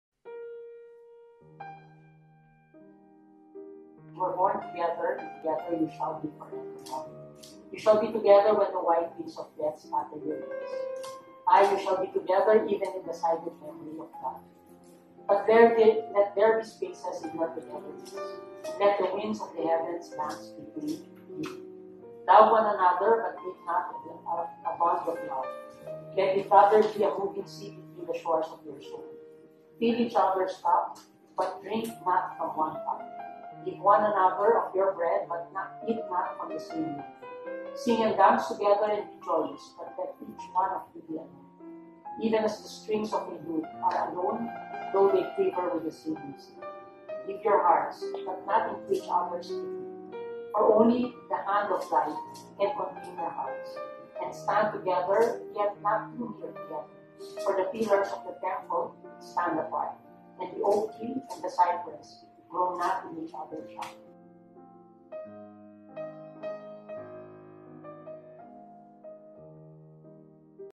Carlos and Camille - On Marriage By Khalil Gibran (as read by Justice Marvic M.V.F. Leonen)
You were born together, and together you shall be forevermore.
You shall be together when the white wings of death scatter your days.
Ay, you shall be together even in the silent memory of God.
But let there be spaces in your togetherness,
And let the winds of the heavens dance between you.

Love one another, but make not a bond of love:
Let it rather be a moving sea between the shores of your souls.
Fill each other’s cup but drink not from one cup.
Give one another of your bread but eat not from the same loaf.
Sing and dance together and be joyous, but let each one of you be alone,
Even as the strings of a lute are alone though they quiver with the same music.

Give your hearts, but not into each other’s keeping.
For only the hand of Life can contain your hearts.
And stand together yet not too near together:
For the pillars of the temple stand apart,
And the oak tree and the cypress grow not in each other’s shadow.